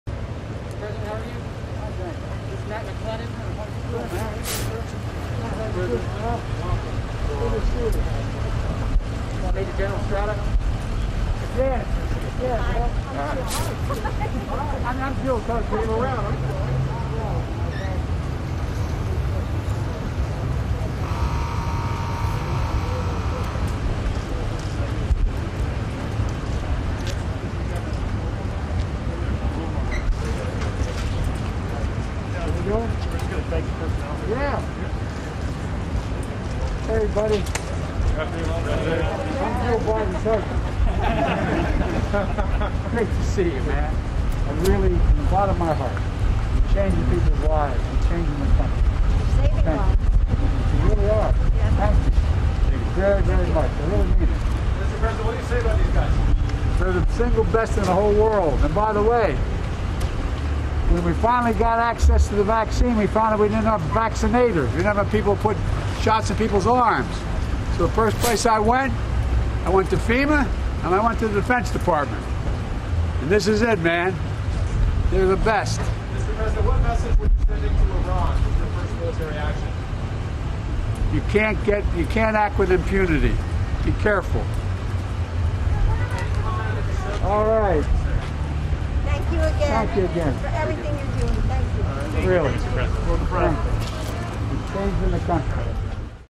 Remarks: Joe Biden Meets Troops at NRG Stadium Vaccination Site in Houston - February 26, 2021
Joe Biden meets with troops staffing the NRG Stadium vaccination site in Houston on February 26, 2021 Uploaded to YouTube for archival purposes by Factba.se